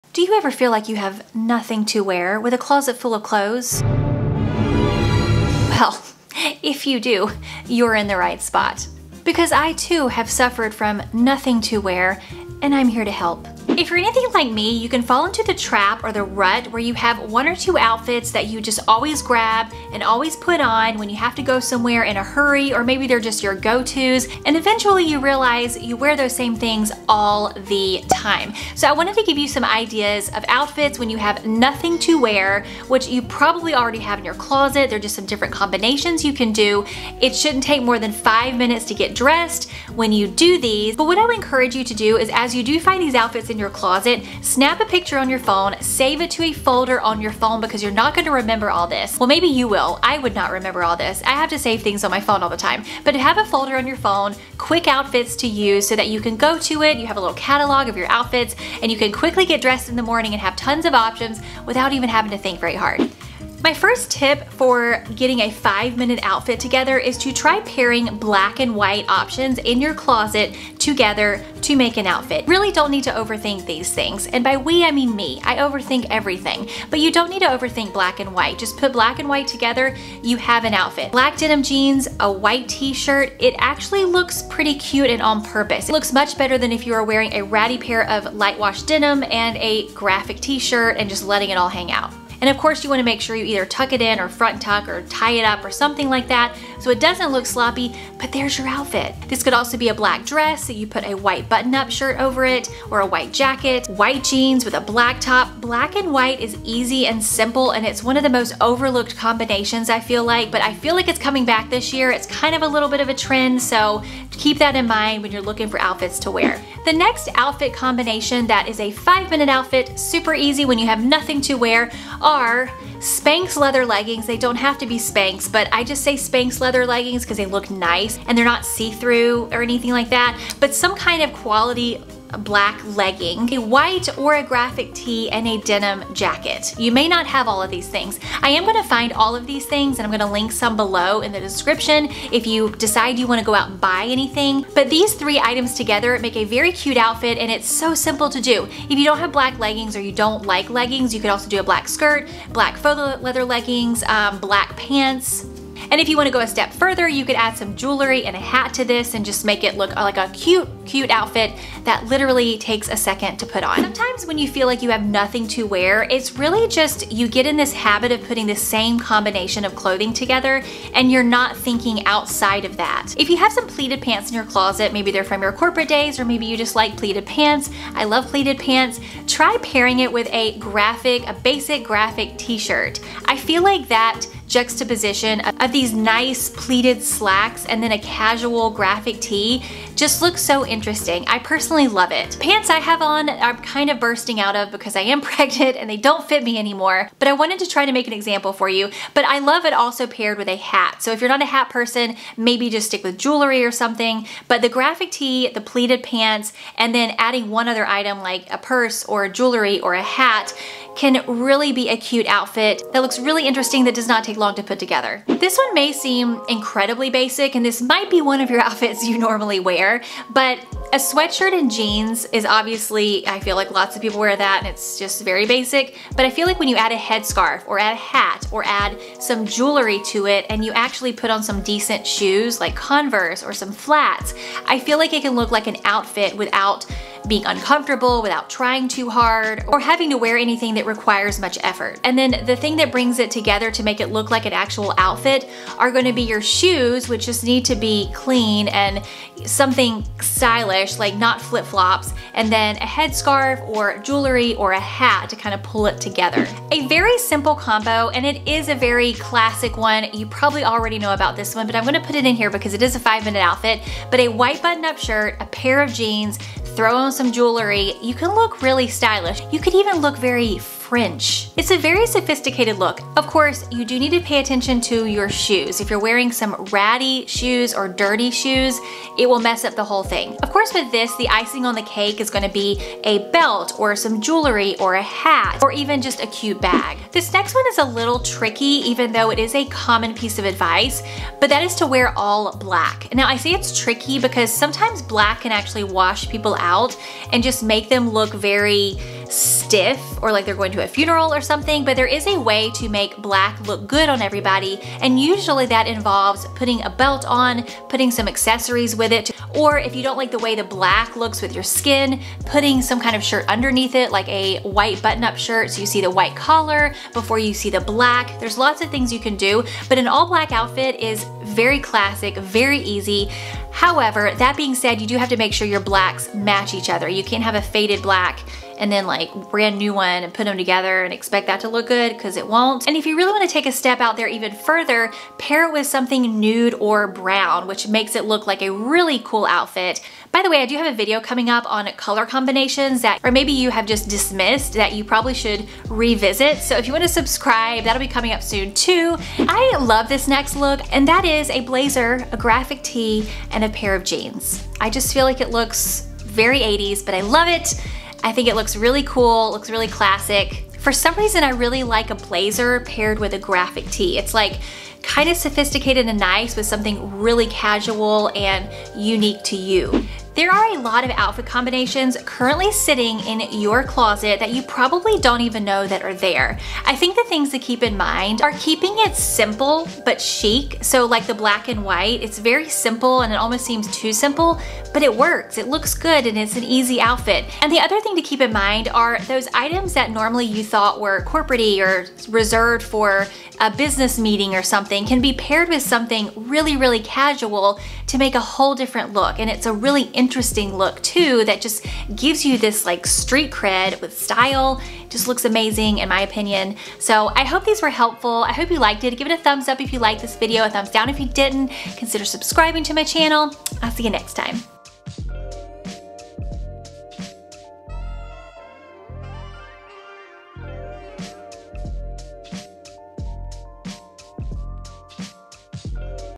Nothing to Wear Outfits *Quick & Easy Outfit Ideas in 5 Minutes!*
Nothing to Wear Outfits *Quick & Easy Outfit Ideas in 5 Minutes!* I've linked a lot of the products I talk about / shown below. This video features some super simple outfit ideas - so easy. that you may have not even realized they are already sitting in your closet! I love easy outfit ideas that are comfortable and do not require much effort. There are so many reasons why you feel like you have 'nothing to wear' (I will probably do a video on them at some point) but one of the reasons is that you haven't experimented enough with the clothes you already own. This video should inspire some ideas of your own.

Items featured in video:
Black & White:
Black Denim Shorts (if I weren't pregnant I would be getting these!)

Basic White Tee
(White tees above would work well too)
Queen Graphic Tee
Be Kind Graphic Tee
Rolling Stones Tee

Graphic Tee / Pants
Dolly Parton Graphic Tee
VOGUE Graphic Tee
(more graphic tees above)

Sweatshirt / Jeans / Hair scarf or Hat
My fav inexpensive sweatshirt
Hair scarf